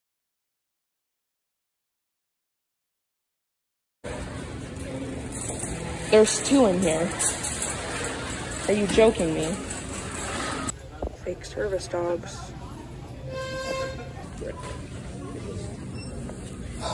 Fake service DOGS! Two fakes in hell mart
So I’m not 100% sure these two were fakes but it’s still very high up there. These dogs were not in a heel and they Couldn’t perform a heel. The one dog was pulling so bad the lady put it in the bottom of the Mobility cart. Once we were leaving and they saw my dog both lunged out but not in a dramatic way, They didn’t walk towards us but they also didn’t viciously lunge at us it was kind of in between. Dixie couldn’t care less, Which I’m proud of but if these dogs were aggressive my dog could’ve missed an alert. If they were real service dogs they shouldn’t be in public because my state doesn’t grant service dogs in training access unless they’re considered fully trained service dogs under ADA So basically they have to be fully obedience trained, fully potty trained, and fully task trained. These dogs cannot even sit much less heel. So I doubt these were real service dogs and if their service dogs in training the owners are breaking the law by bringing their service dog in training in Walmart￼￼